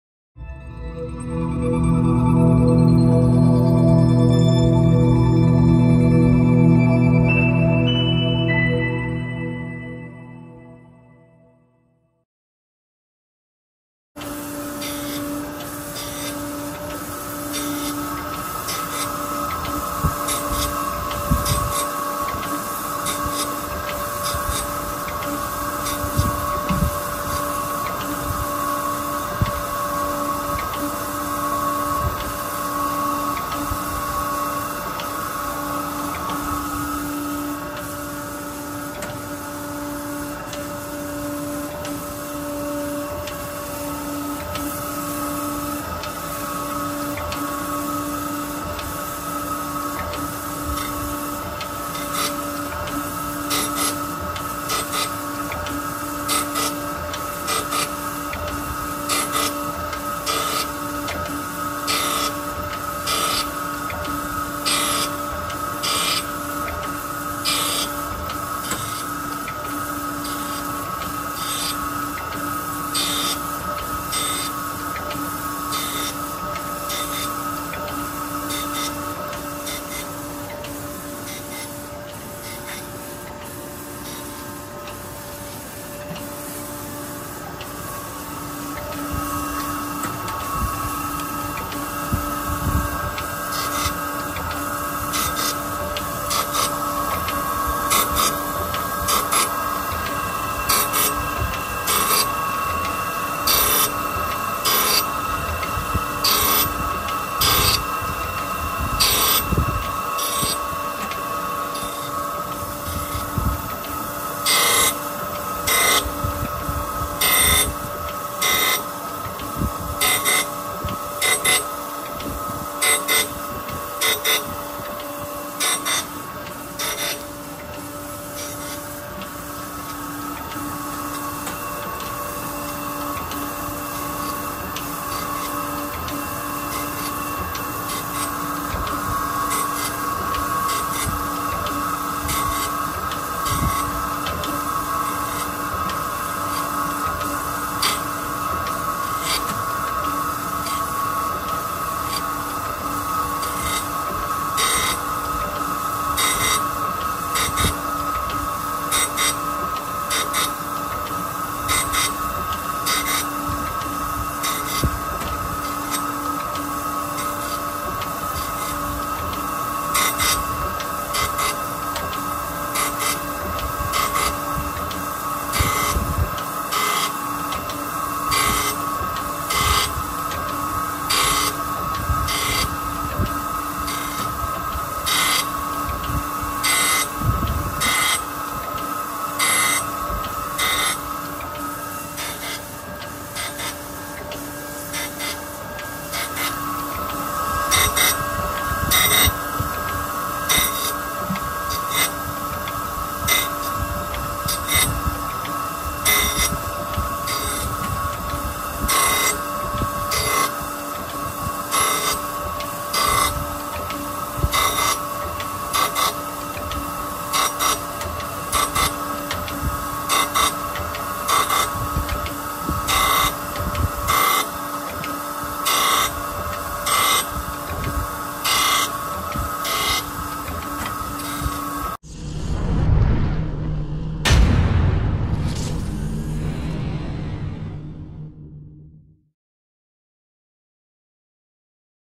12" X 24" OKAMOTO ACCUGAR 124N HYDRAULIC SURFACE GRINDER. STOCK # 0788524.
MODEL 124N ACCUGAR
CAPACITY 12" X 24"
MAXIMUM UNDER WHEEL 13.7"
HYDRAULIC TABLE FEED 1 TO 100 FPM
LONGITUDINAL TABLE TRAVEL 26"
AUTO CROSS TRAVEL 13.6"
LONGITUDINAL FEEDS INTERMITTENT ... 0.02" TO 0.8"
LONGITUDINAL FEEDS CONTINUOUS 4 TO 40 IPM
CROSS FEED RATE INTERMITTENT 0.02" TO 0.8"
CROSS FEEDS RATE CONTINUOUS 4 TO 40 IPM
INCREMENTAL DOWN FEED OF HEAD YES
AUTO FEED PER TABLE STROKE 0.0015" TO .0001"
AUTO FEED PER SADDLE REVERSE 0.0015" TO .0001"
RAPID FEED RATE 19 IPM
GRINDING WHEEL SIZE 12" X 1-1/2" X 5"
OVER THE WHEEL DRESSER YES
CHUCK VARIABLE HOLD 12" X 24" X 3"
SPINDLE MOTOR 5 HORSEPOWER
HYDRAULIC MOTOR 2 HORSEPOWER